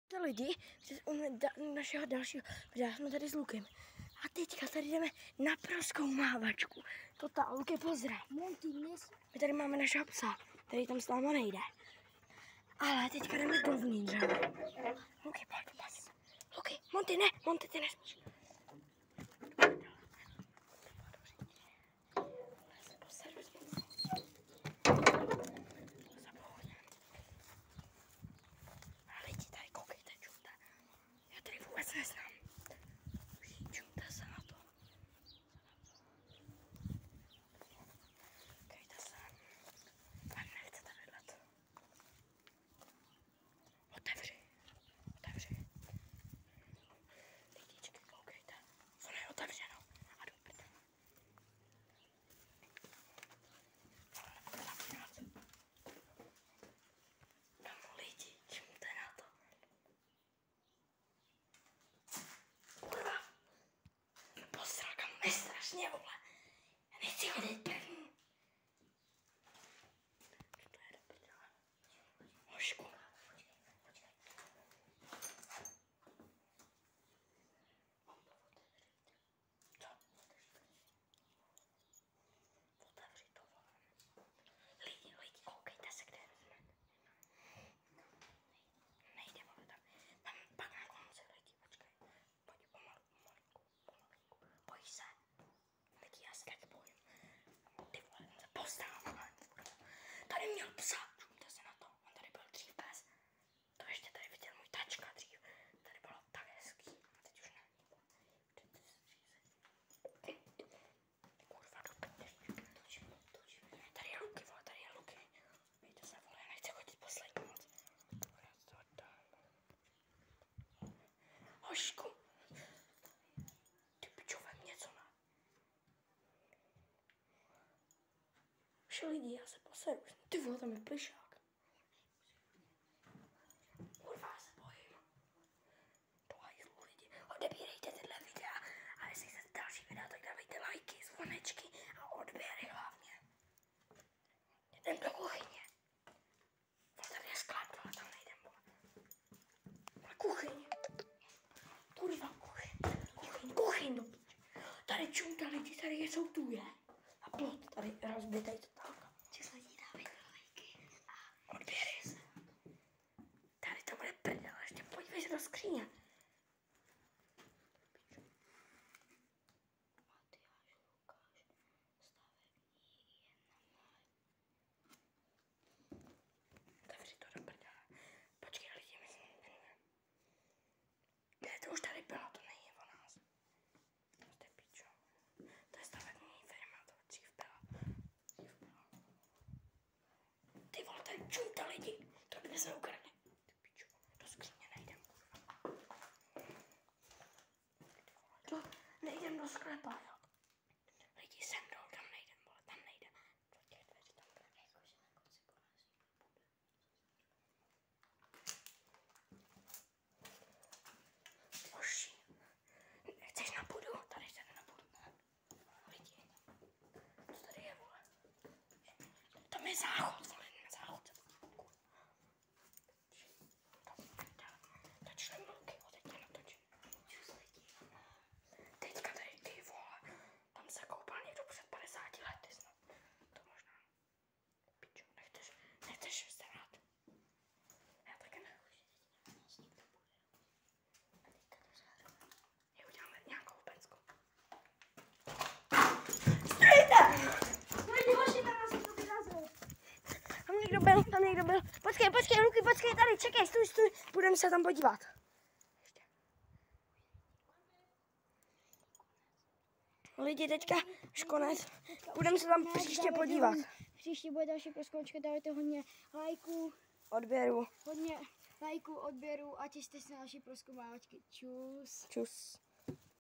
18. července 2023(4)
srašidelná prozkoumávačka v děsivém domě děsivé!!!